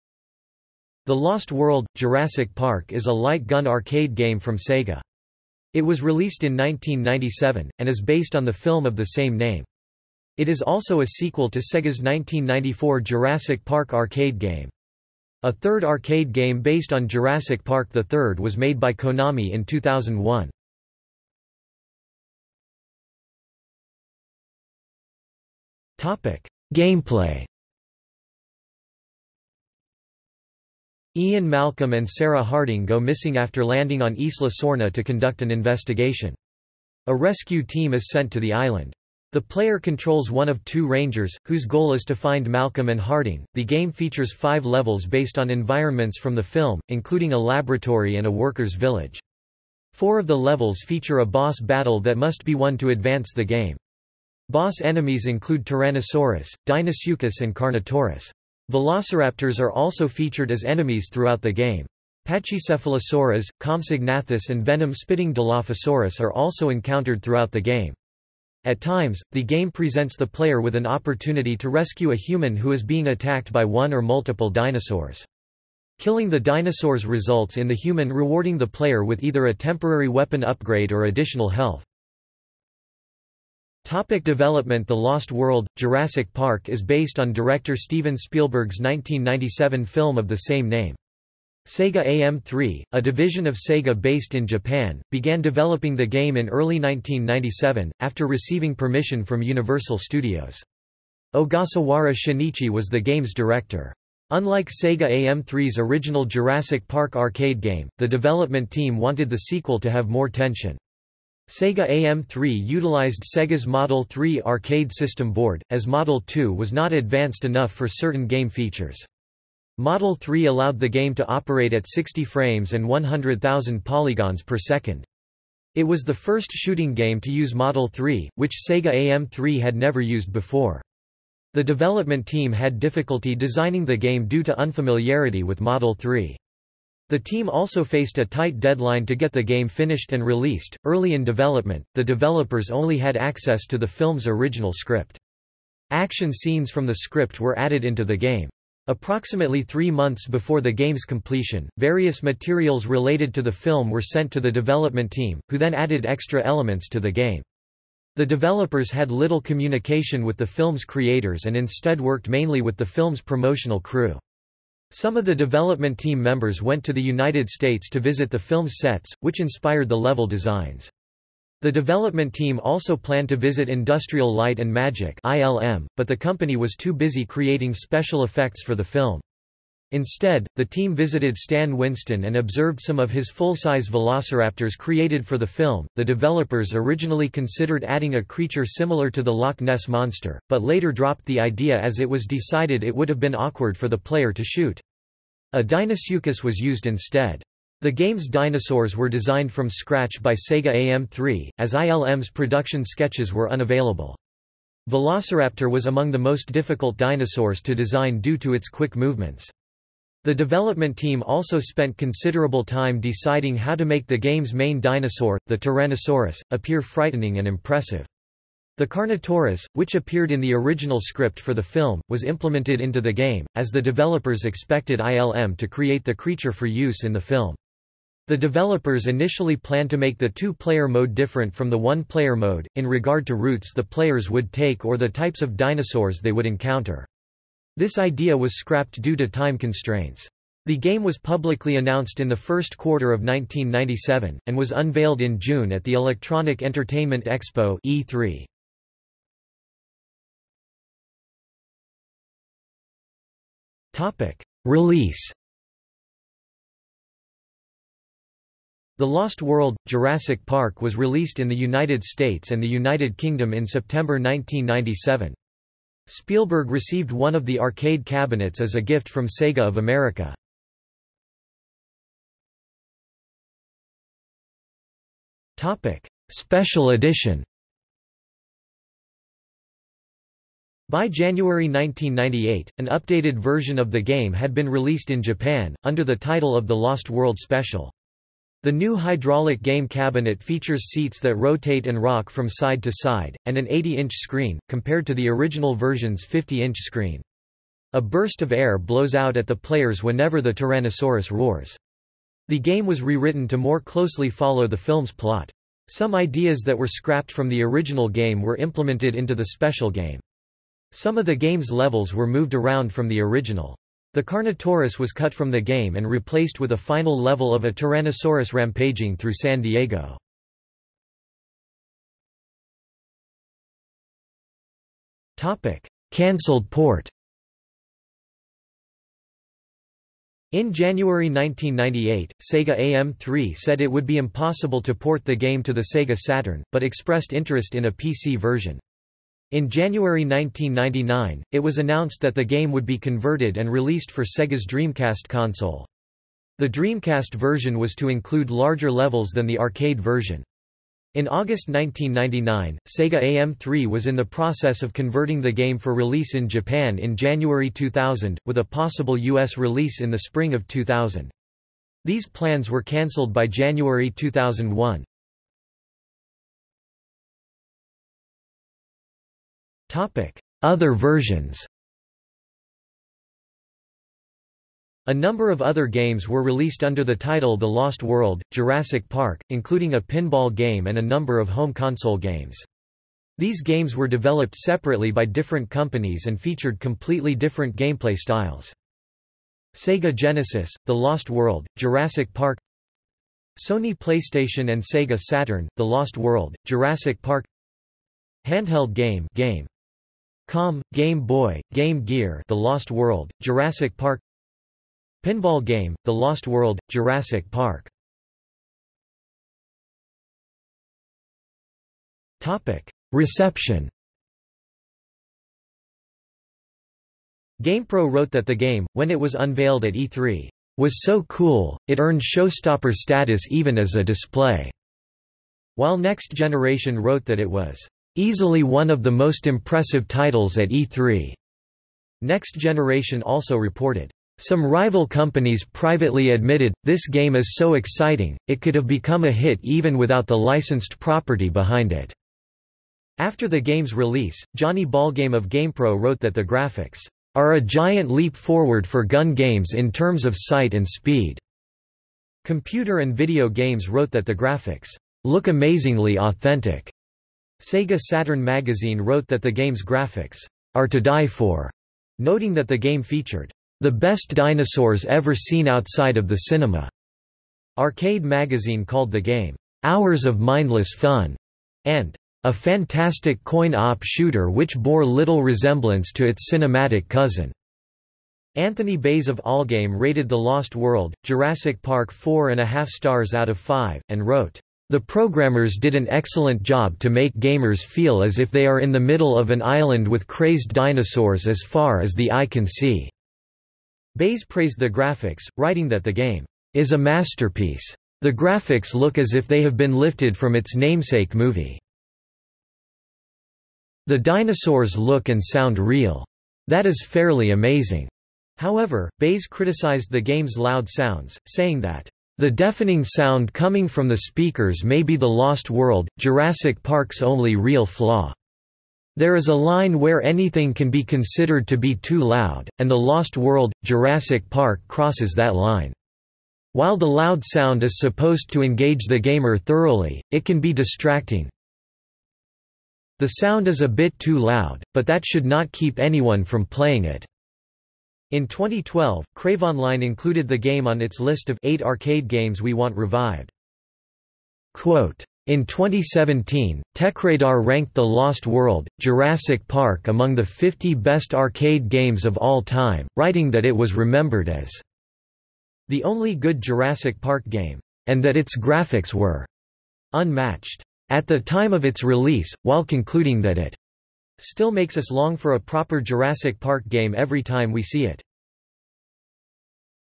The Lost World: Jurassic Park (arcade game) | Wikipedia audio article
00:00:22 1 Gameplay
00:01:23 2 Development
00:04:10 3 Release
00:04:32 3.1 iSpecial/i edition
00:05:22 3.2 Cancelled port
00:06:06 3.3 Other versions
00:06:48 4 Reception

- Socrates

SUMMARY
The Lost World: Jurassic Park is a light gun arcade game from Sega. It was released in 1997, and is based on the film of the same name. It is also a sequel to Sega's 1994 Jurassic Park arcade game. A third arcade game based on Jurassic Park III was made by Konami in 2001.